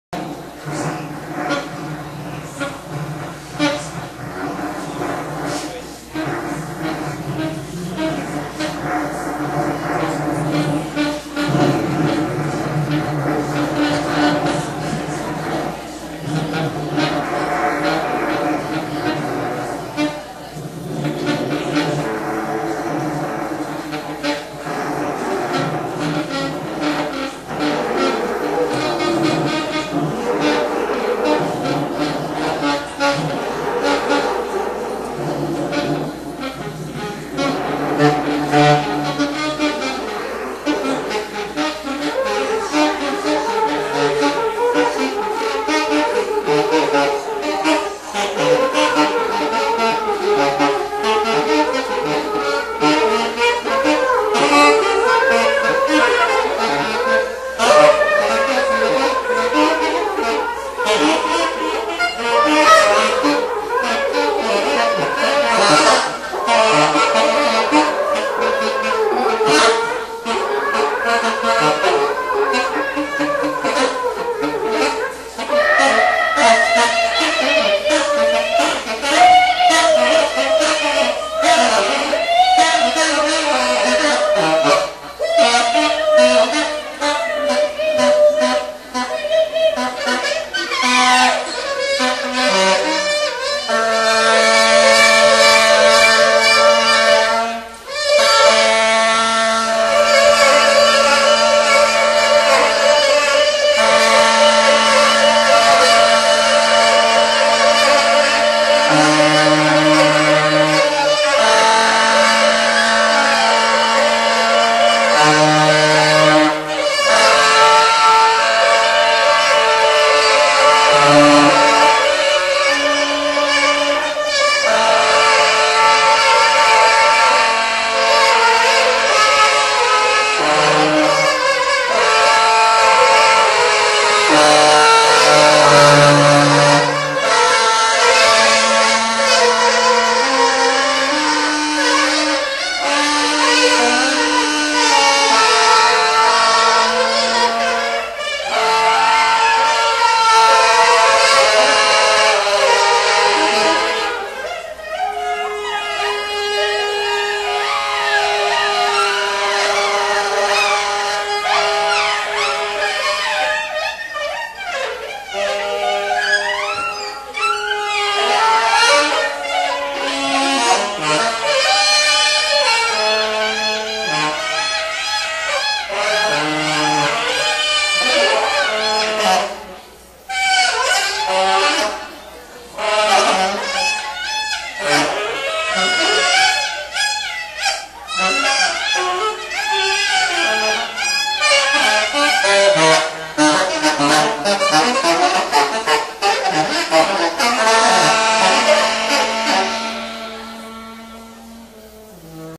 Jack Wright & Andreas Stehle - Excerpt ( 1987 Free Impro - Noise -Experimental )
Great and weird free improvisation-experimental music from Sound Of Pig label back in the golden 80's.